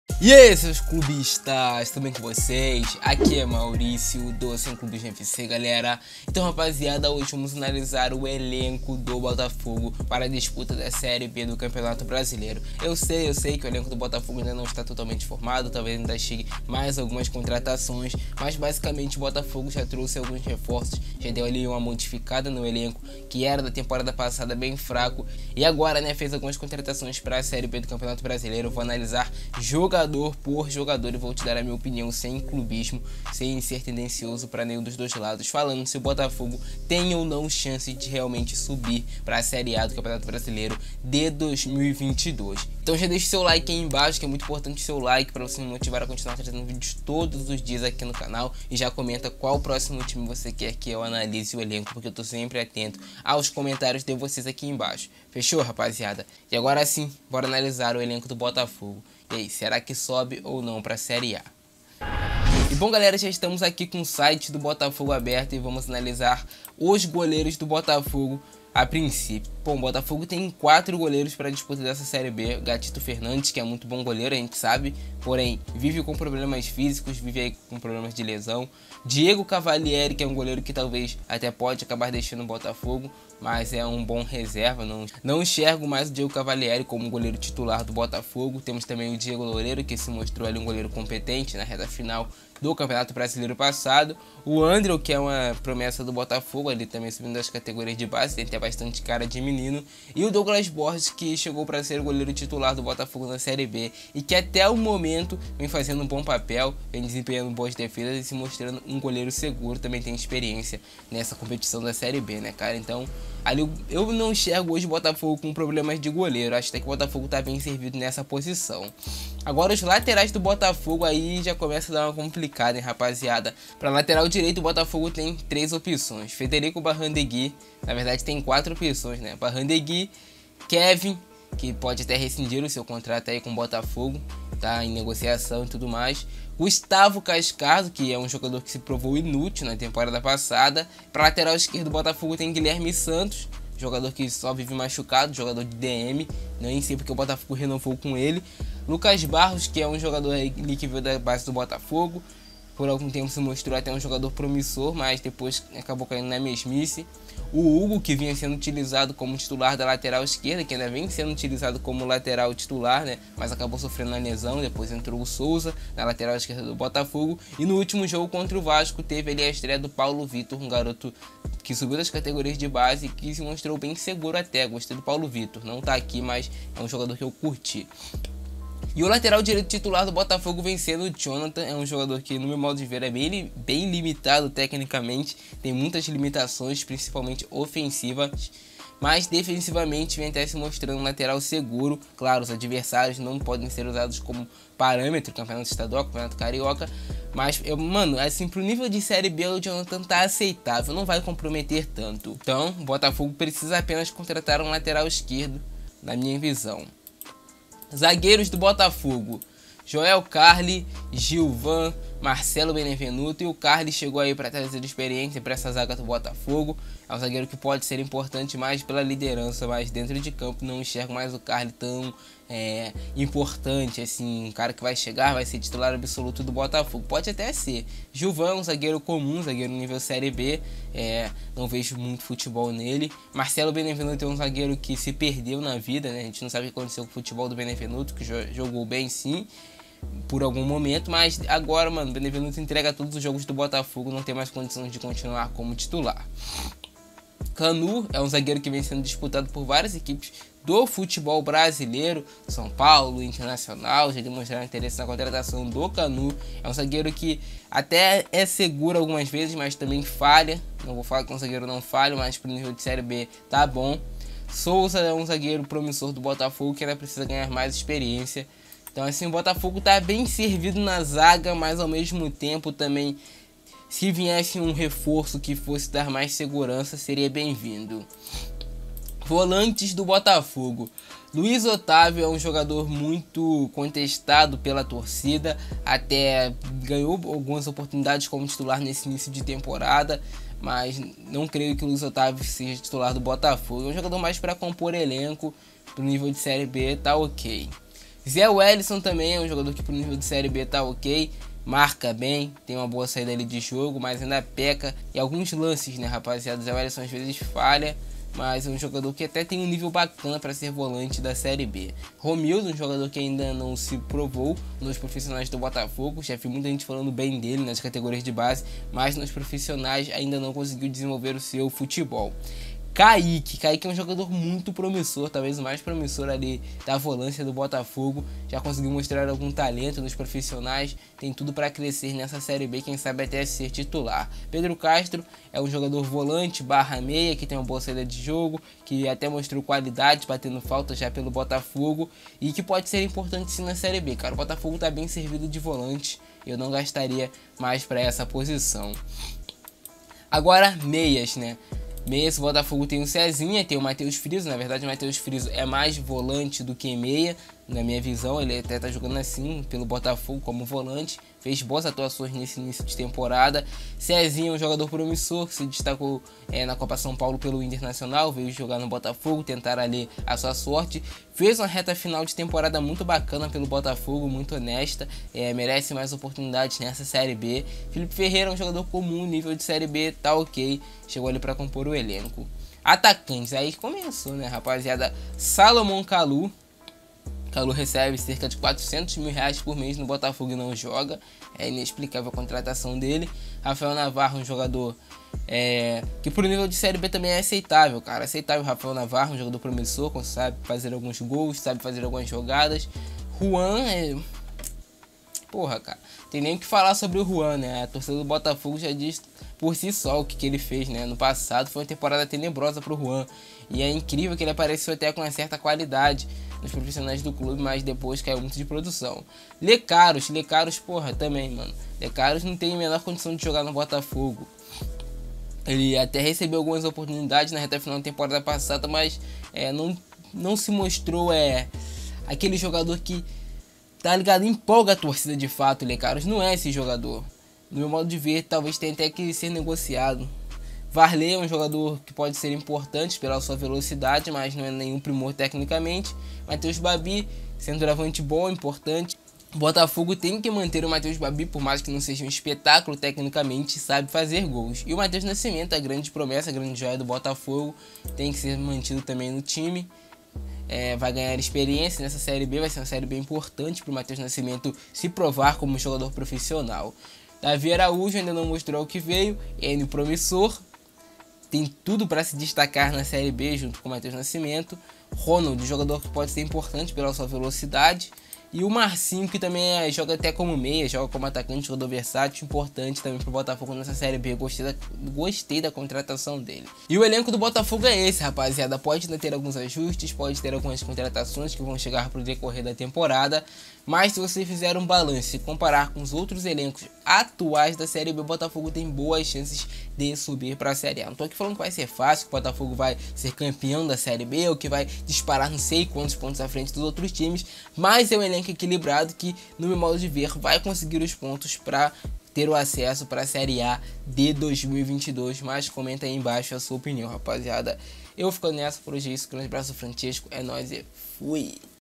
ANALISANDO O ELENCO DO BOTAFOGO PRA SÉRIE B 2021 - DA PRA SUBIR PRA SERIE A?
Então galera hoje temos mais um video aqui no canal analisando elencos pra temporada 2021, e dessa vez vamos analisar o elenco do botafogo pra temporada 2021, sera que com esse elenco da pro botafogo voltar pra serie a em 2022? com esse atual elenco da pro botafogo ser campeão da seire b 2021?

LOJA